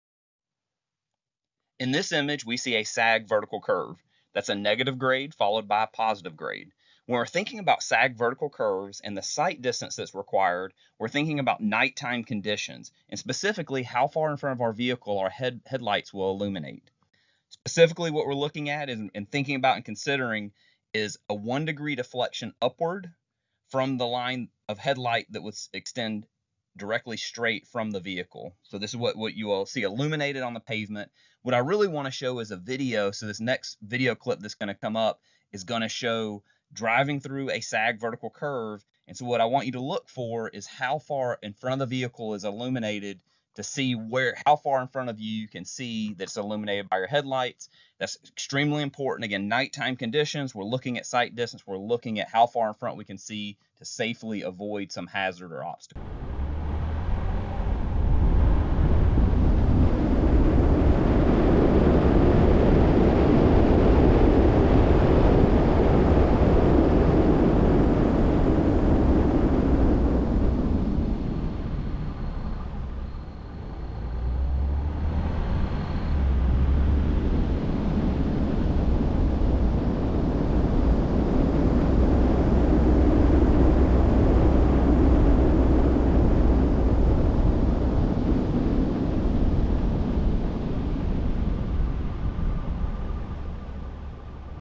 Sag Vertical Curve Sight Distance Demonstration [Nighttime Conditions]
1:00 Start of First Drive Through
1:14 Start of Second Drive Through (from opposite direction)